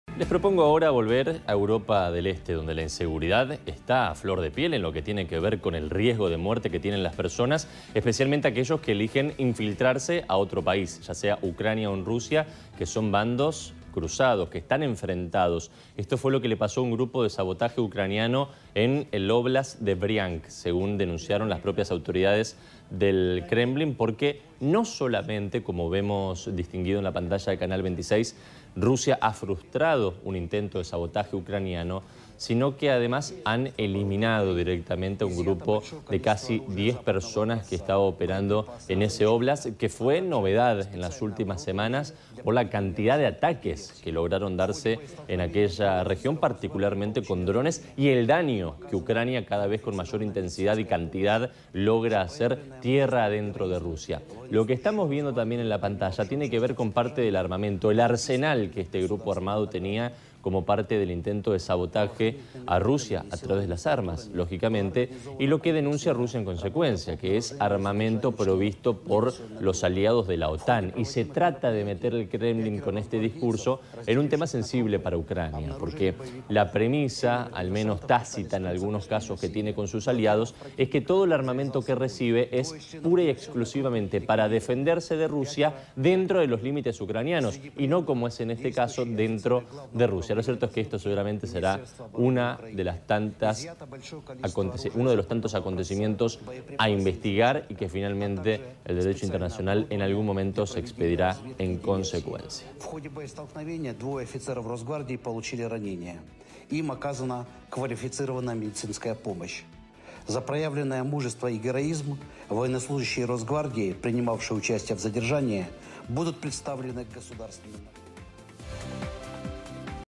GUERRA RUSIA-UCRANIA | El Ejército ruso eliminó a un grupo de sabotaje ucraniano en Bryansk
👉 Fuerzas del Servicio Federal de Seguridad (FSB) de Rusia eliminaron un grupo de sabotaje al servicio de Ucrania en la región rusa de Briansk.